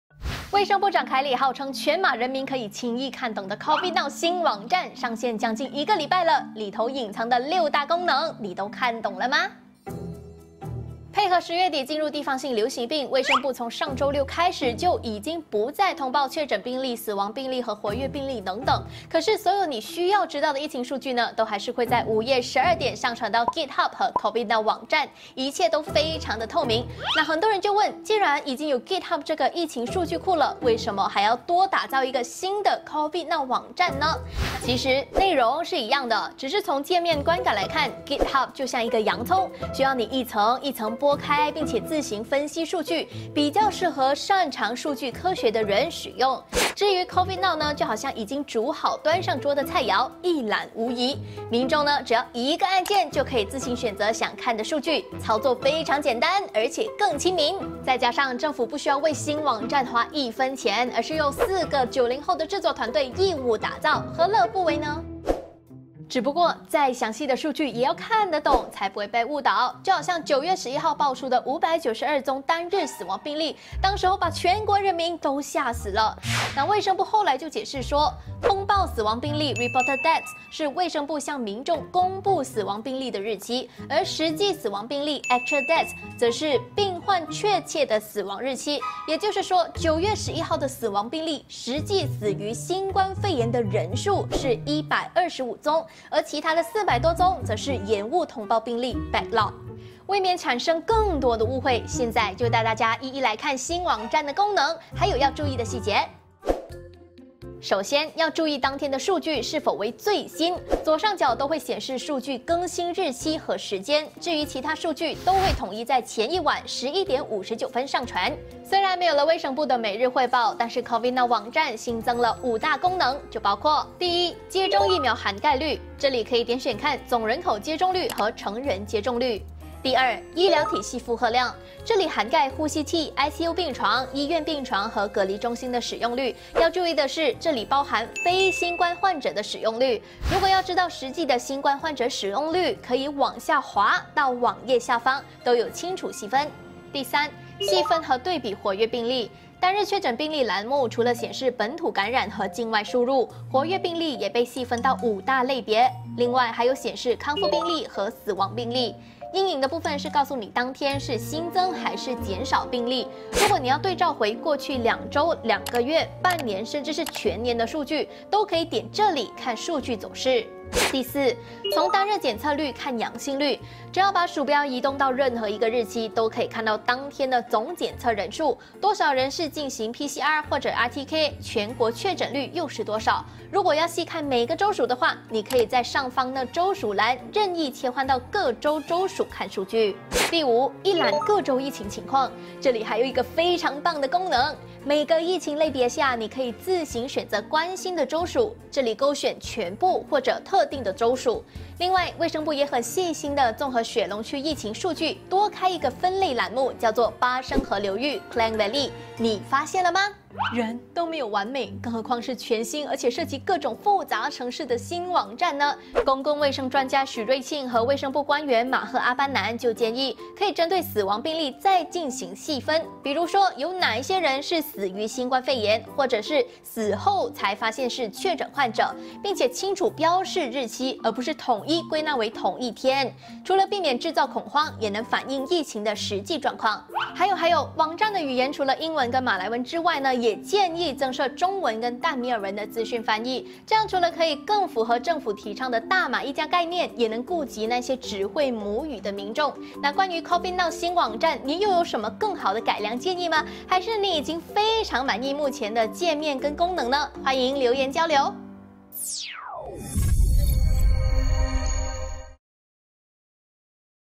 ▌热点影音 ▌COVIDNOW 6大功能一览1个按键掌握全国疫情状况
卫生部刚推出的COVIDNOW新网站才上线，就因为死亡病例出现2种数据而导致全国“虚惊一场”。虽然说操作简单、界面明瞭，但当中隐藏的6大功能你发现了吗？看影片一起来搞懂数据，避免再被傻傻牵着走~